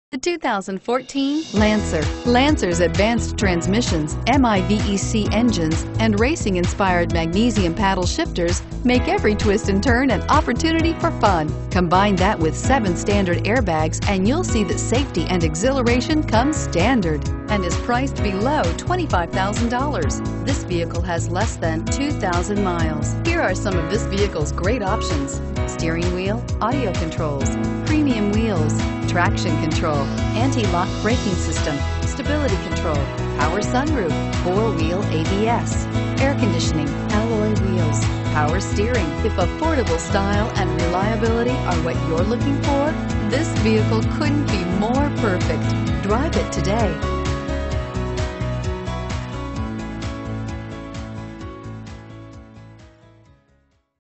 2014 Mitsubishi Lancer Puyallup WA M010419
2014 Mitsubishi Lancer Sportback ES FWD

2001 N. Meridian

Check out THIS custom ride!  This Mitsubishi Lancer is FIT with some great looking goodies - in addition to the  factory options, which include the Deluxe Package, Exterior Package and Popular Equipment Package... this one has tinted windows, smoked lights, and an awesome matte black high-performance wheel and tire package... with a $2,495 value on top of the MSRP of $22,440, this really sets this vehicle apart!  Check it out today!      Pricing includes all available rebates: Bonus Cash Rebate $1500, Military Rebate $500, Loyalty Rebate $500